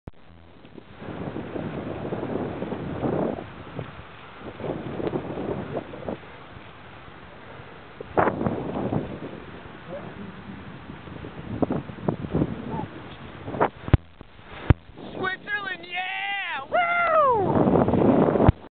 Konstanz to Zürich
This was a wild ride along Lake Constance in Germany and Switzerland.